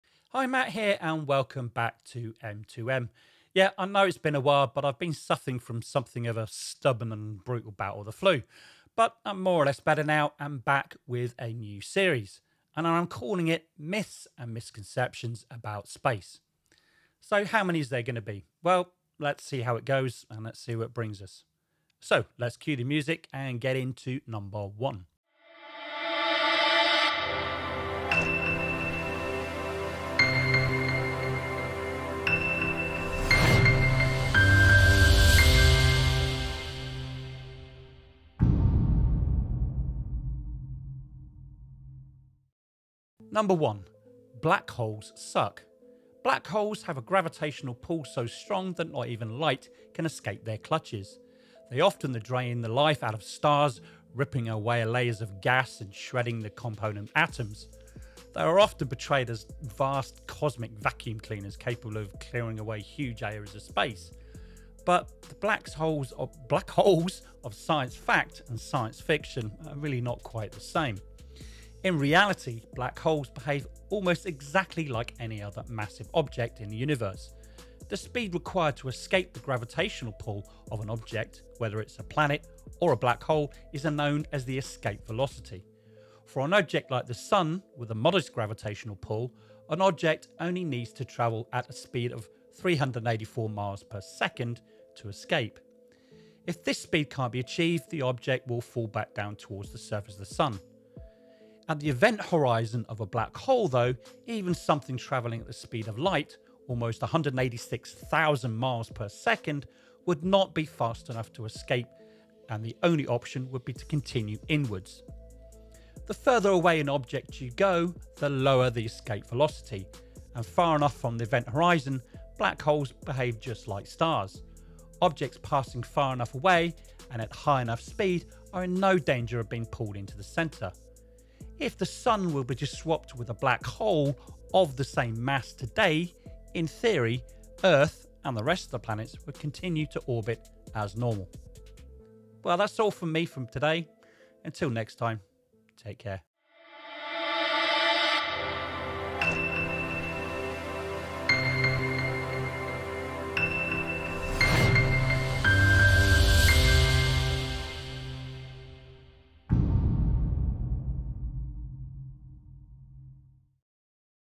Myths and Misconceptions about space#1 | Black Holes Suck?
Black holes are one of the most incredible objects in the universe, and misconceptions about them abound. In this video, we'll clear up some of the most common misconceptions about black holes and help you understand them a little better.